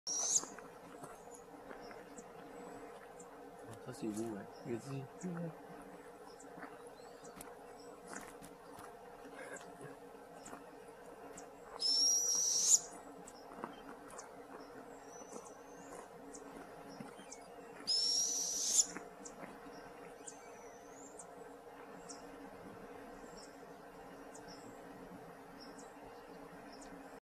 La Chouette Épervière de l'Auberge ! (Hawk Owl)
Chouette Épervière filmée en HD à l'Auberge de Montagne des Chic-Chocs en Gaspésie, Réserve faunique de Matane
Images avec autorisation de Yvan Verenka.